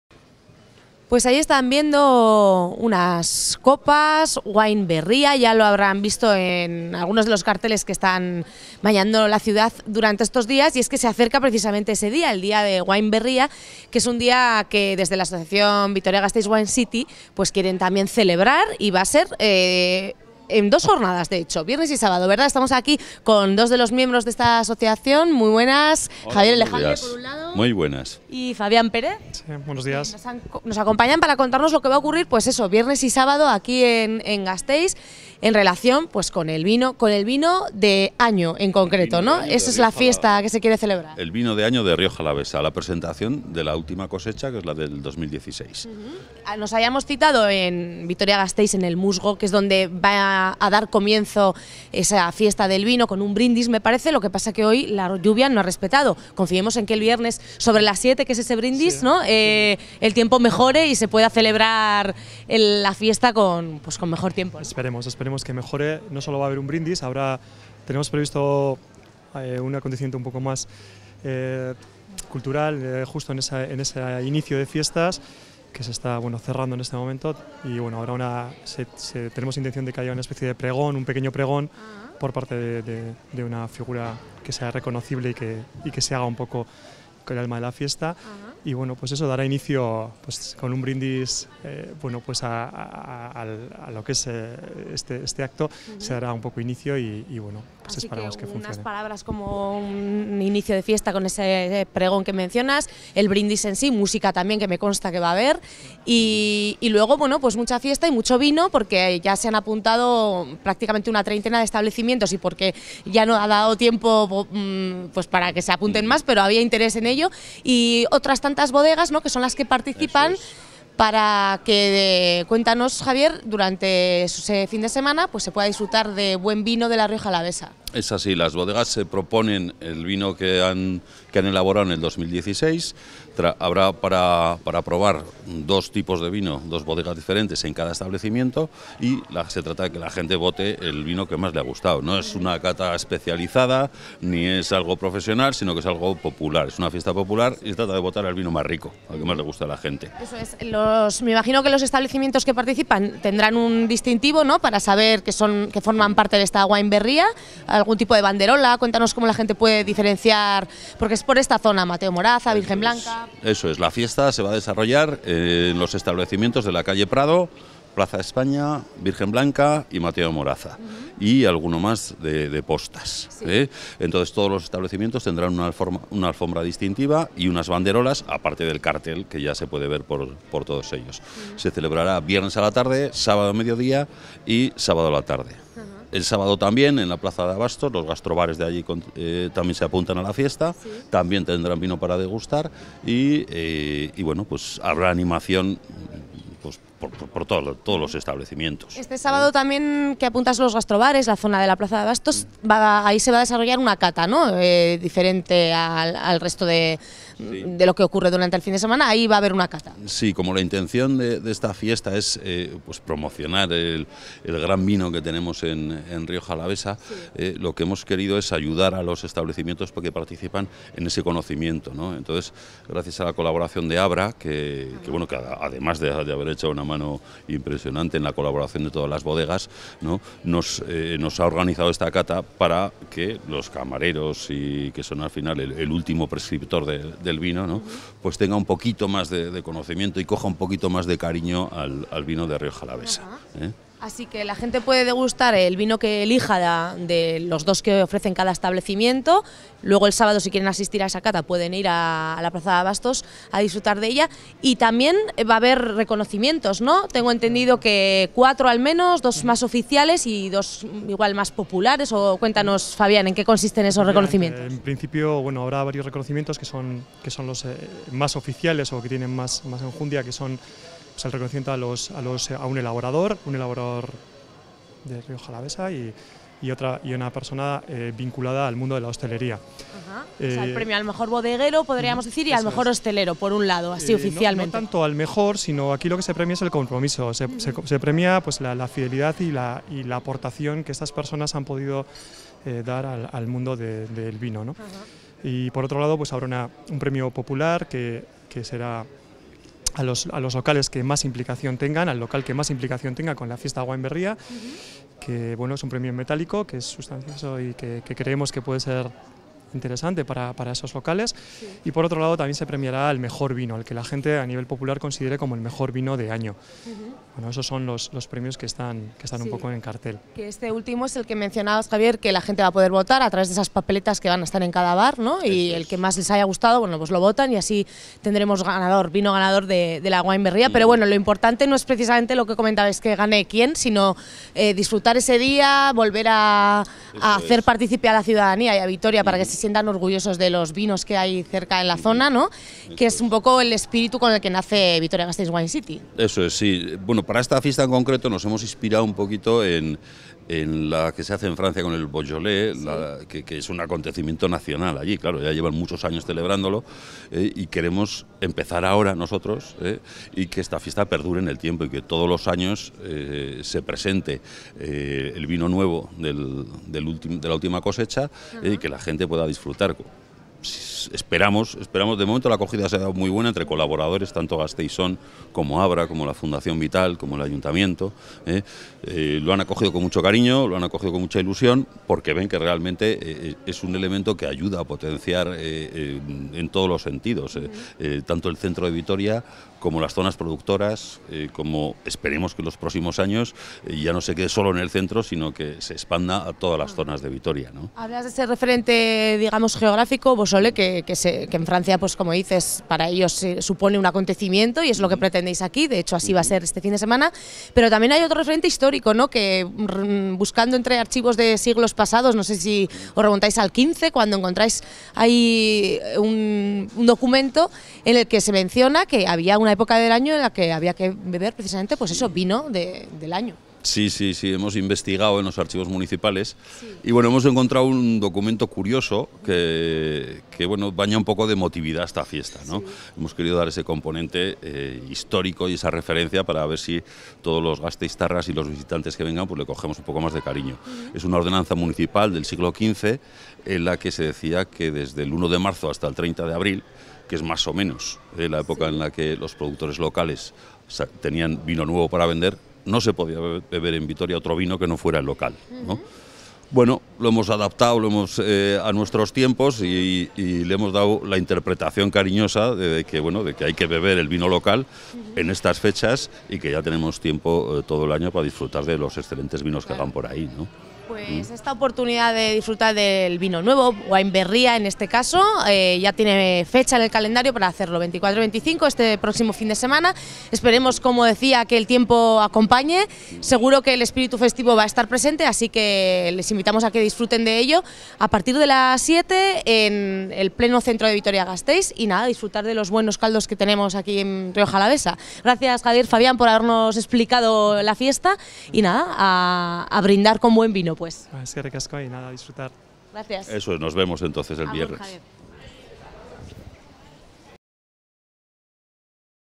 VITORIANEANDO Wine Berria
Vitorianeando conocemos todos los detalles de la feria "Wine Berria", I fiesta del vino joven de Rioja Alavesa en Vitoria-Gasteiz.